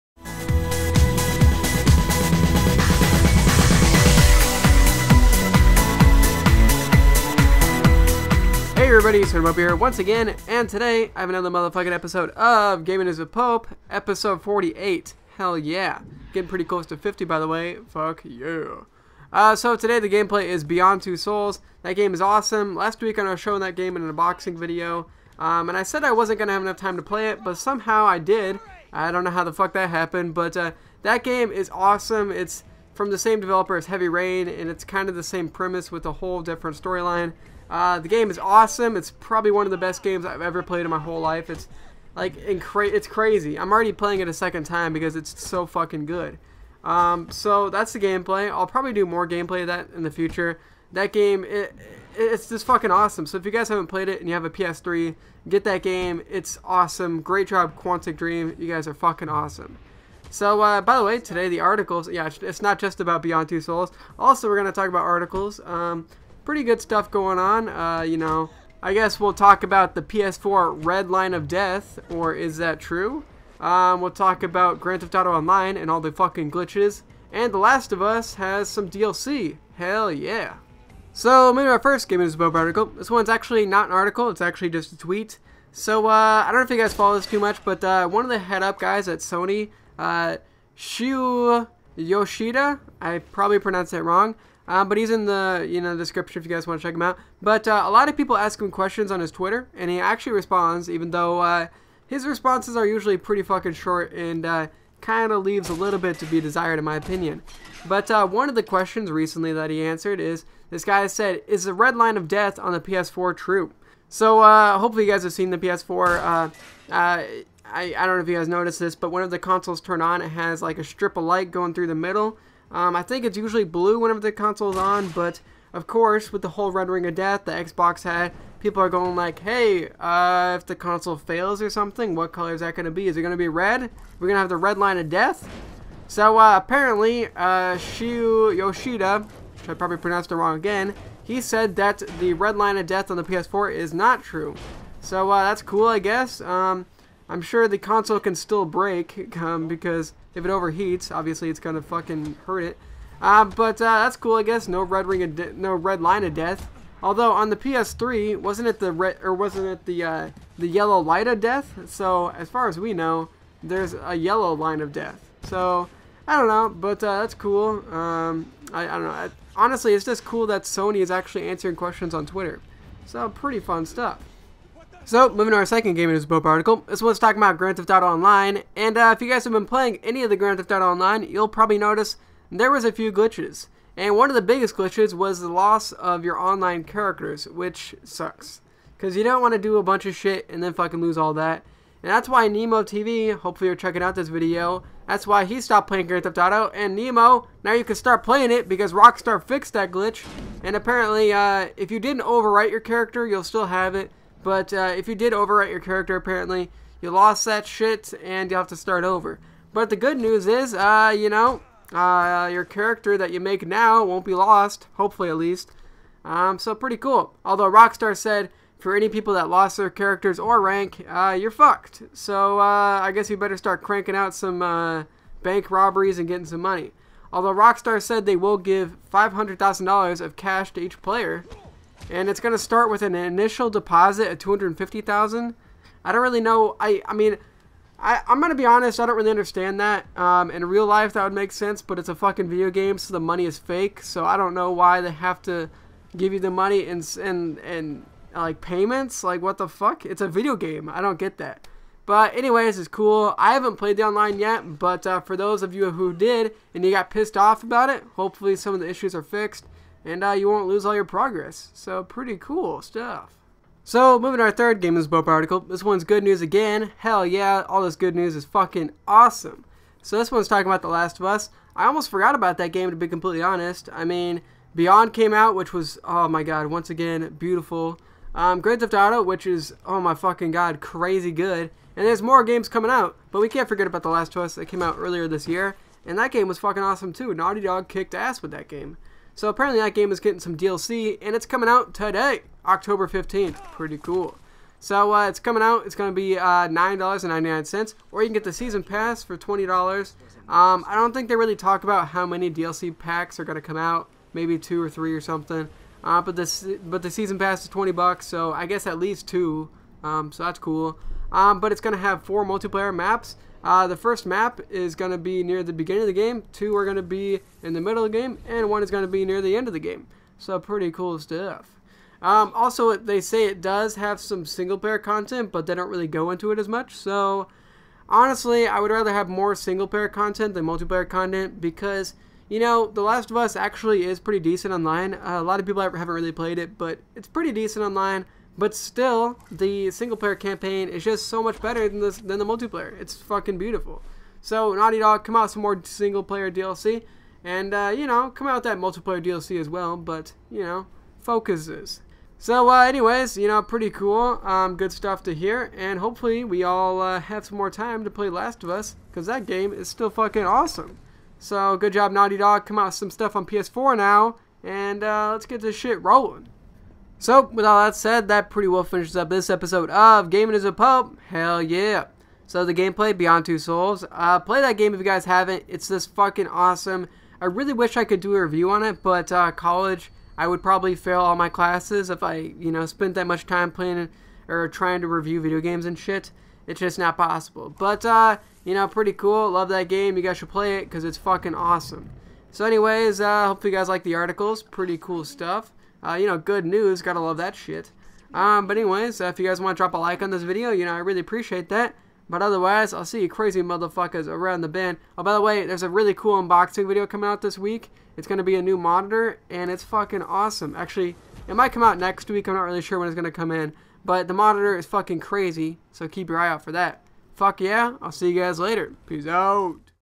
Gaming News with Pope - GTA Online Glitches Fixed - The Last of Us gets DLC!
I got some really great news today. Oh, and the best game ever made: Beyond Two Souls.

I really appreciate it when you guys Like the video. Go ahead, do it ;)

The Last of Us gets some DLC!